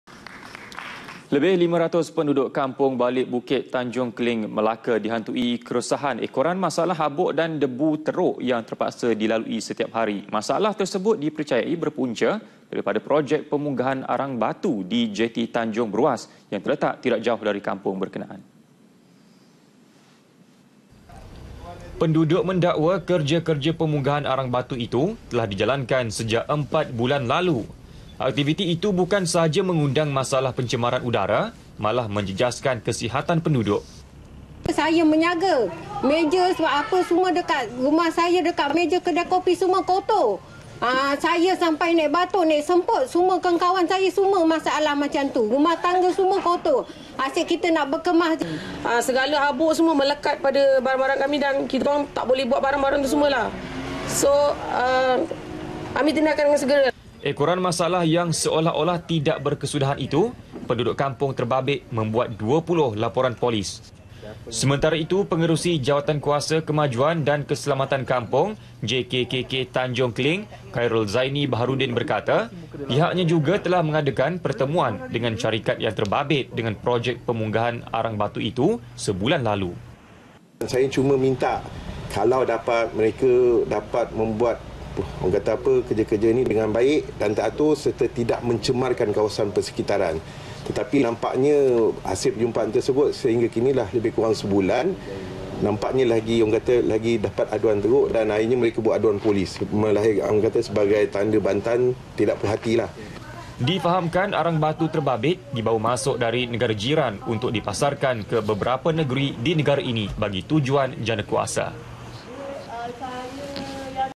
AKTIVITI PEMUNGGAHAN ARANG BATU - PENDUDUK DAKWA BERDEPAN MASALAH DEBU TERUK [23 MEI 2017]
Lebih 500 penduduk kampung Balik Bukit, Tanjung Kling, Melaka dihantui keresahan ekoran masalah habuk dan debu teruk yang terpaksa dilalui setiap hari.

Masalah tersebut dipercayai berpunca daripada projek pemunggahan arang batu di Jeti Tanjung Beruas yang terletak tidak jauh dari kampung berkenaan.